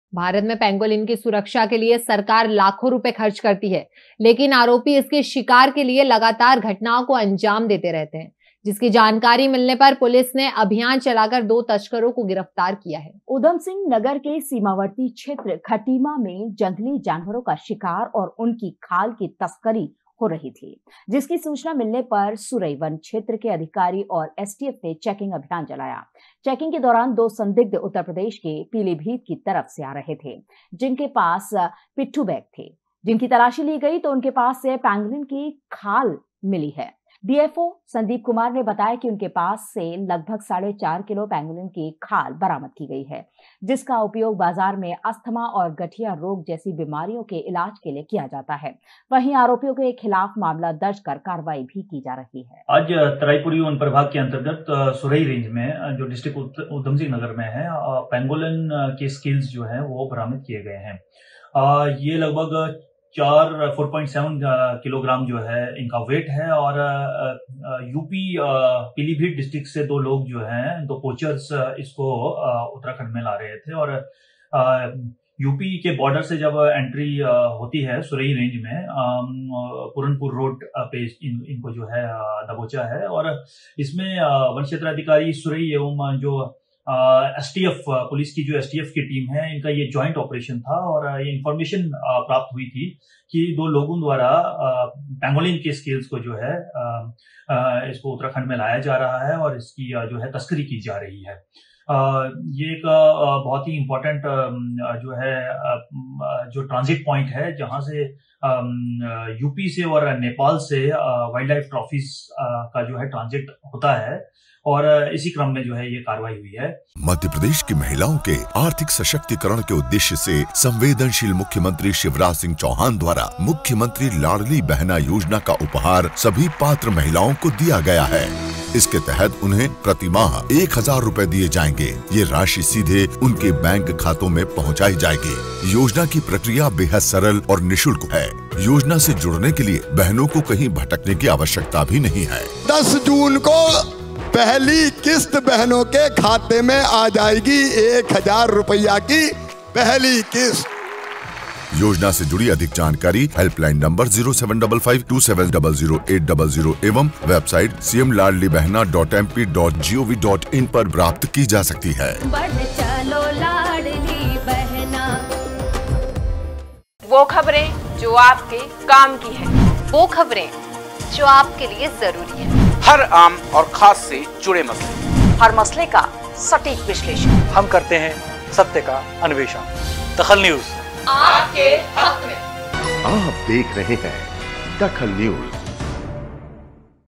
DAKHAL NEWS पैंगोलिन की खाल के तस्करी गिरफ्तार
पैंगोलिन की खाल के तस्करी  गिरफ्तार
खटीमा में  पकड़ी गई पैंगोलिन की खाल